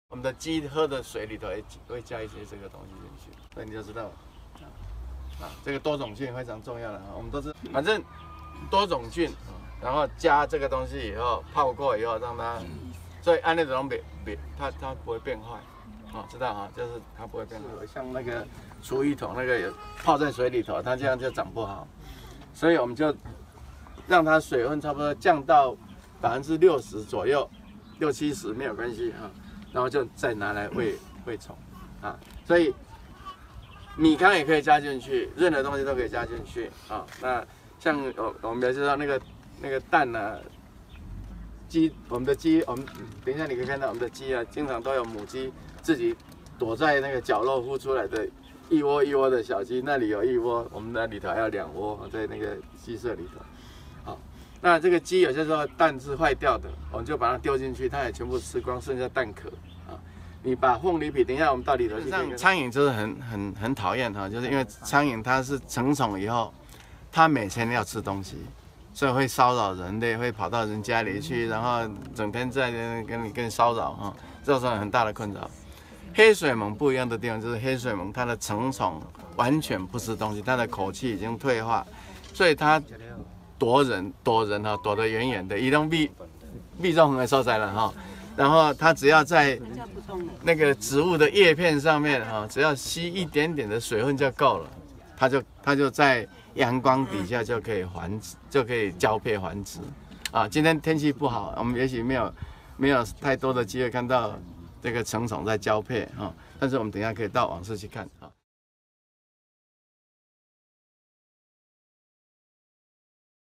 毫無懸念！廚餘養黑水虻 1.5小時就上手
本報日前獨家報導前明道大學校長陳世雄用廚餘、廢豆渣養黑虻幼蟲再拿來餵雞，引起廣大迴響，陳世雄今天特別開班授課，他課前說這是「1個半小時保證班」，學員們沒一個相信，但真的1.5小時的課程上完，大家真的都學會如何操作。